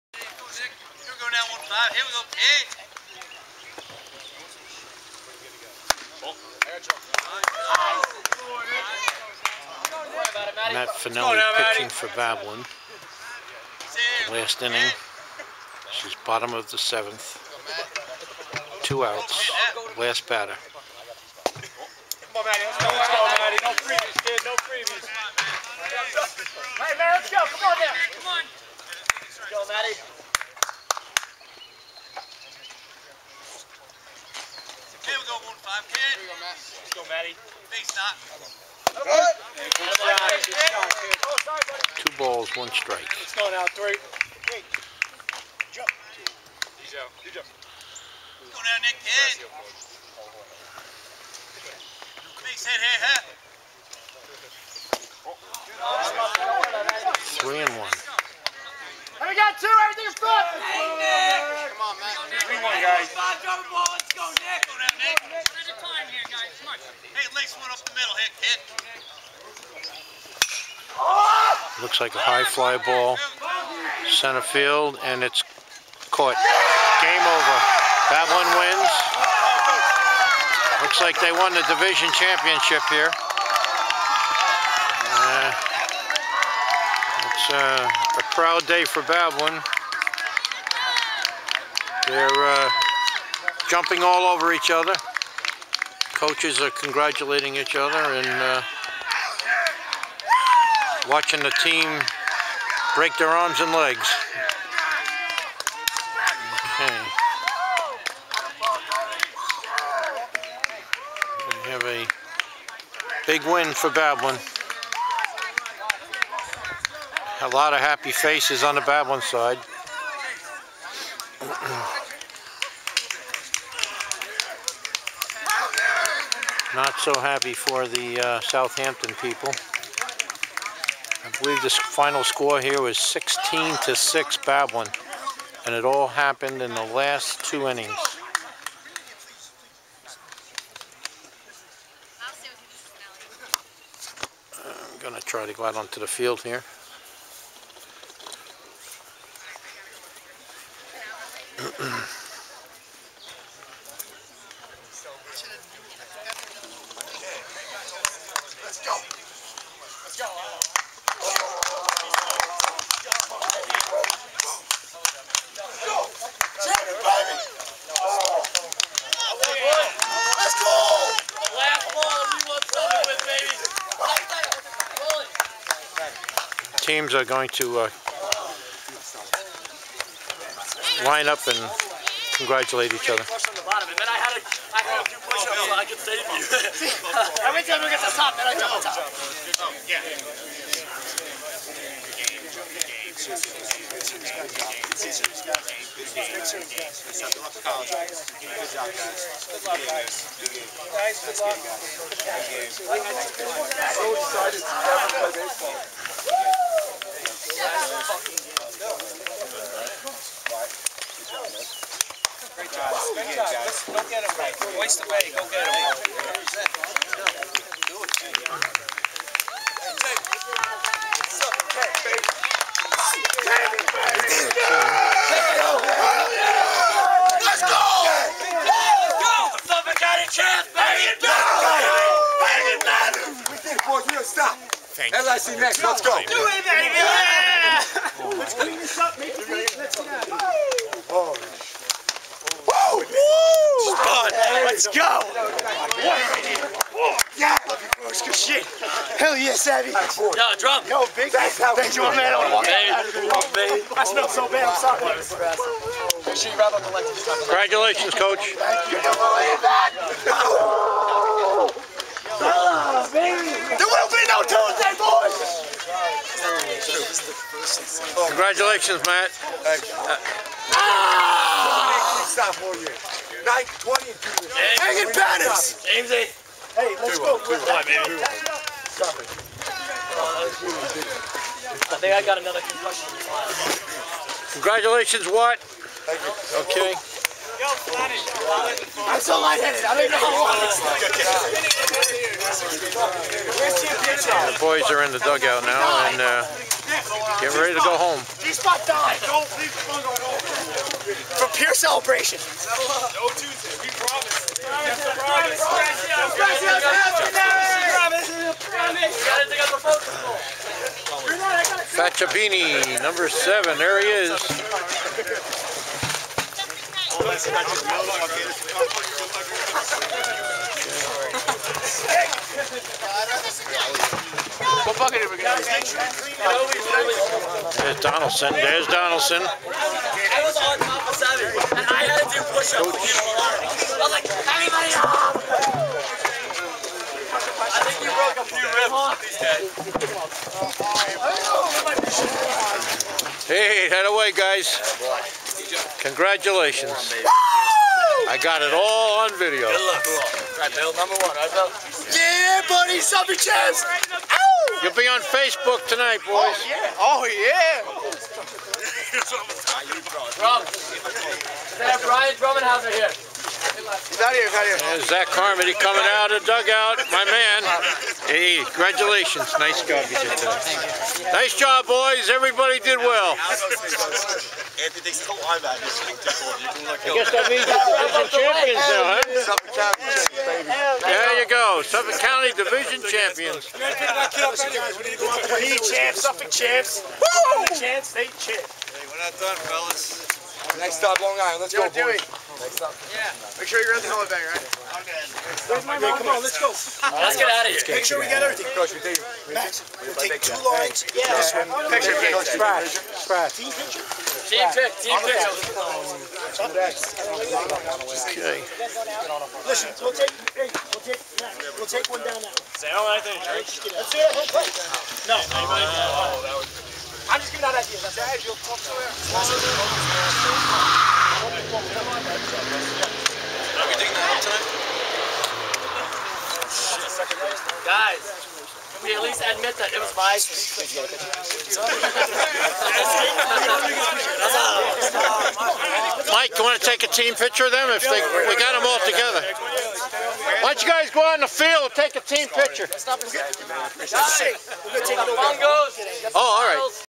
2012 0525 Babylon HS Varsity clinch the Suffolk Co Div Championship against South Hampton H S at So
Babylon High School Baseball won the Suffolk County Title in May 2012. Here is the video of the last out and the celebration that followed.